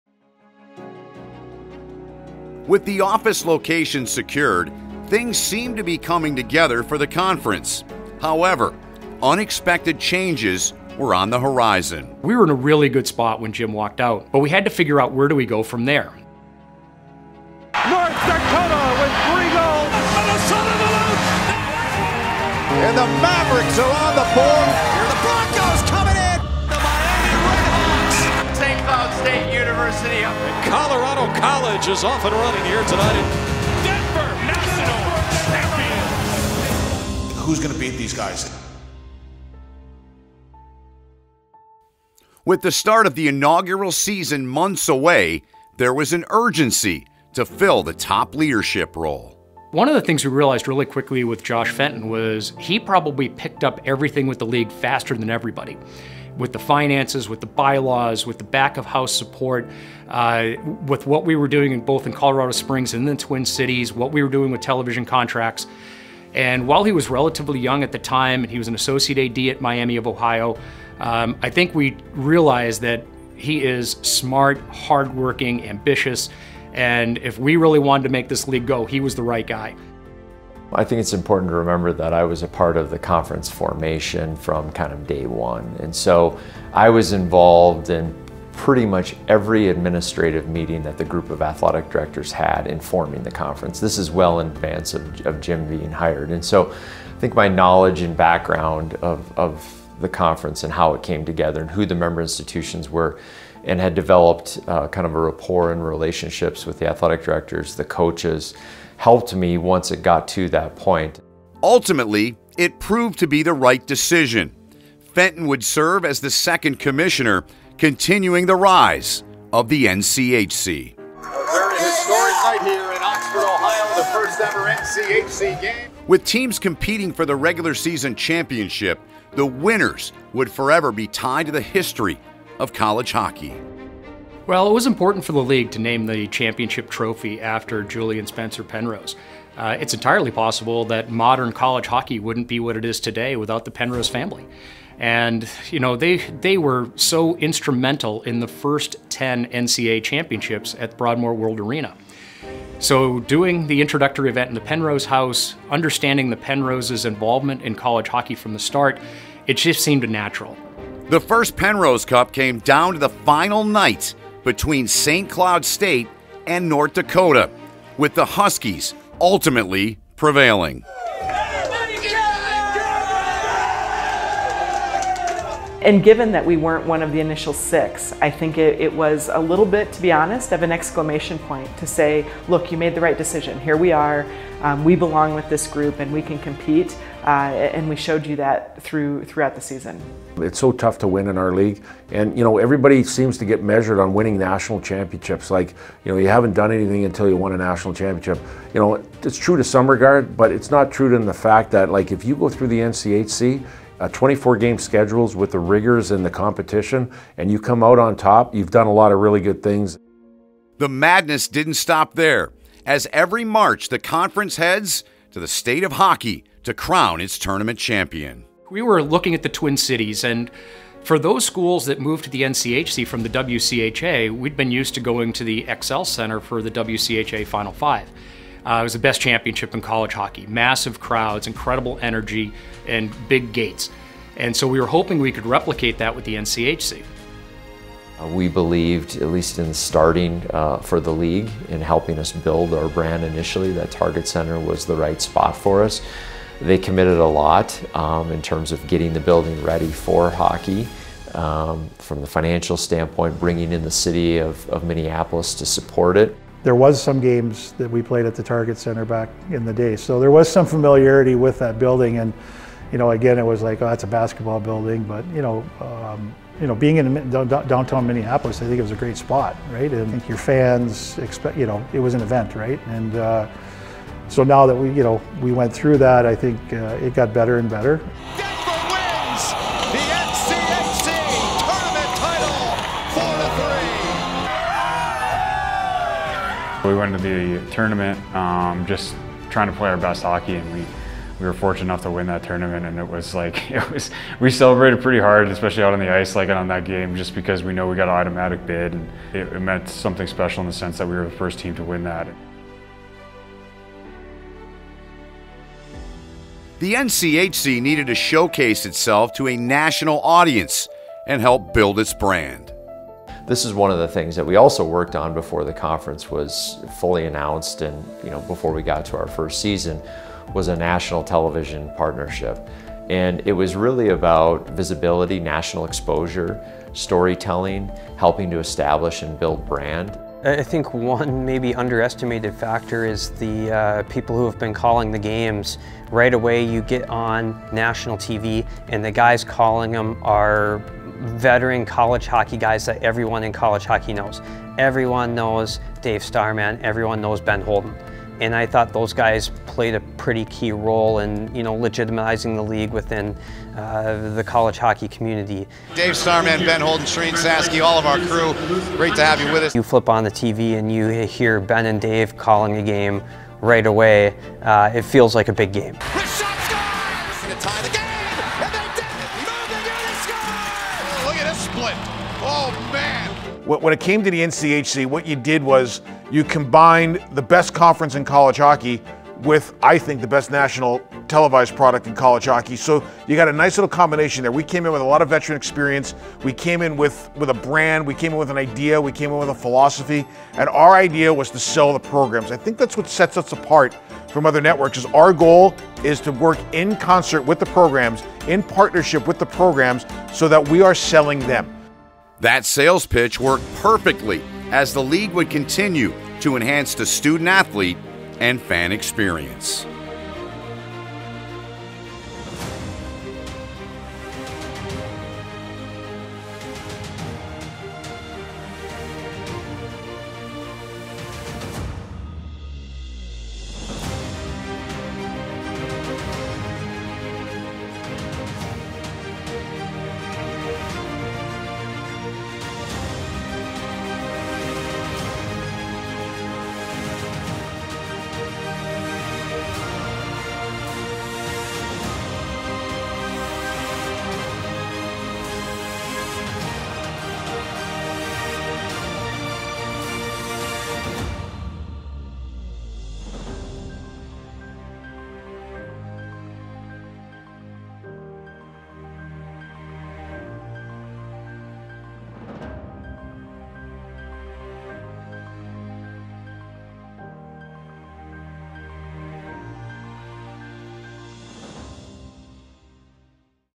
The Rise of the NCHC: A Decade in College Hockey - 'Dropping the Puck' (Episode 2)
The second episode of the NCHCHockey 10 year documentary explores the hiring of a new commissioner, the first Penrose Cup and Frozen Faceoff tournament, partnering with CBSSN, and more!